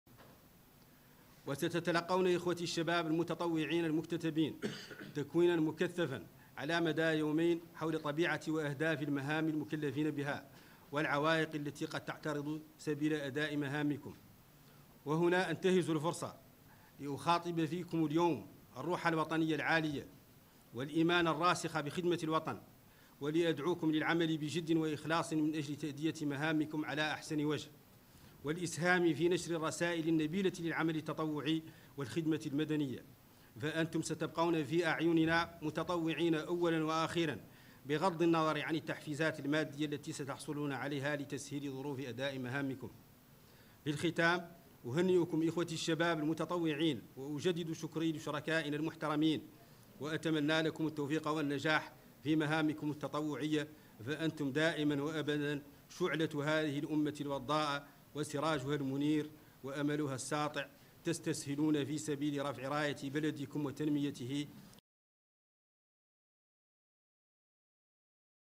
المنسق العام لبرنامج وطننا : سيتلقى المشاركون تكوينا مكثفا على أهداف وطبيعة المهام المكلفين بها
تصريح : أحمدو ولد عبد الرحمن\ المنسق العام لبرنامج وطننا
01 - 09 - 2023
قناة المرابطون - الصفحة الرسمية - 2023